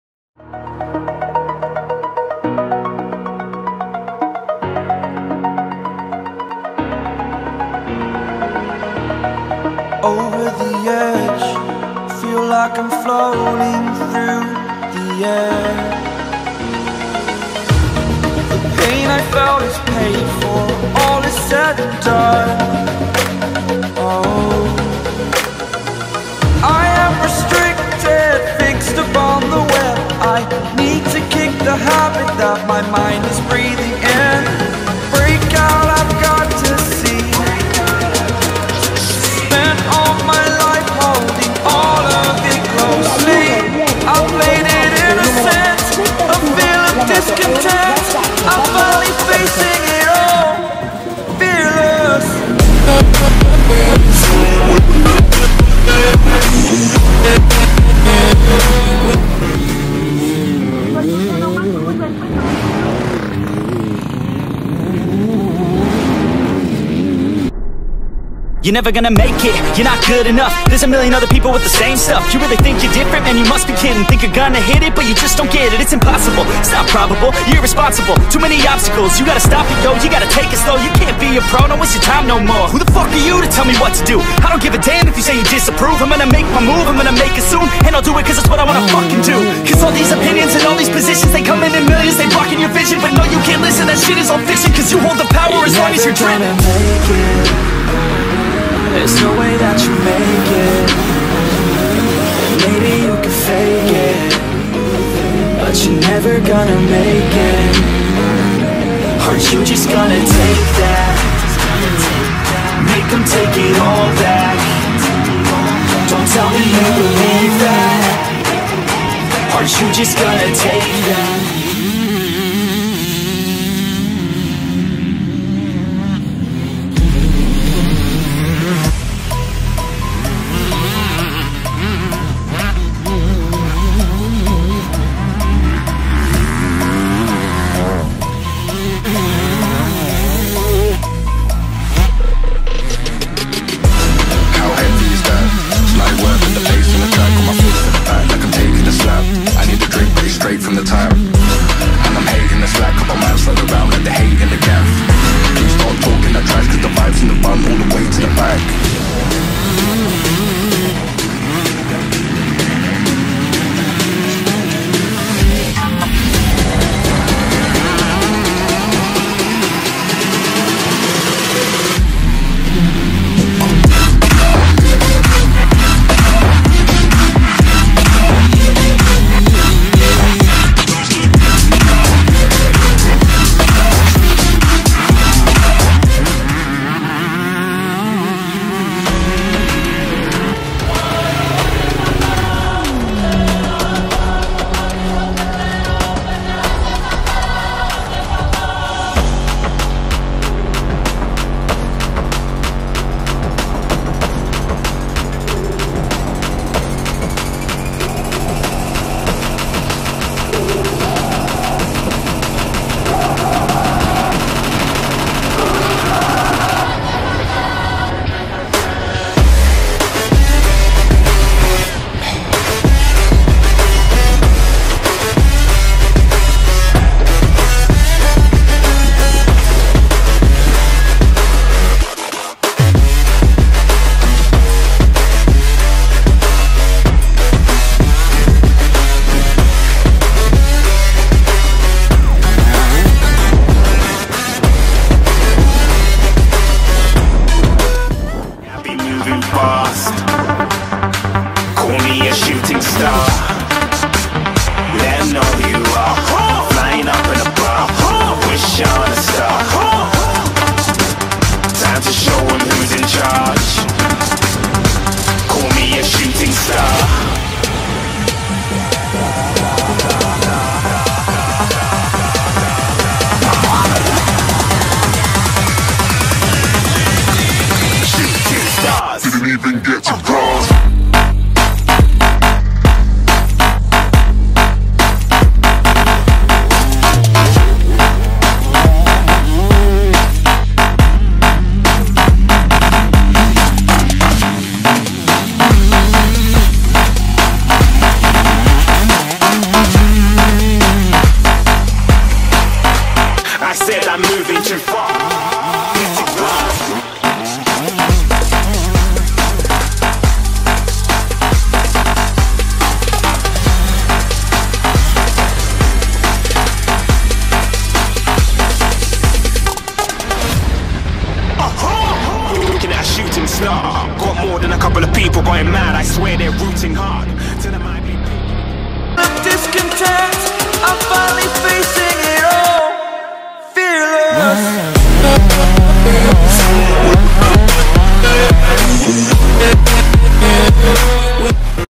Campionato Italiano Enduro 2021
Stagione 2021 terminata , campione italiano 125 senior e vicecampione europeo nella 250 2t, poco meno di 6 minuti per riassumere  la stagione. fatemi sapere cosa ne pensate nei commenti.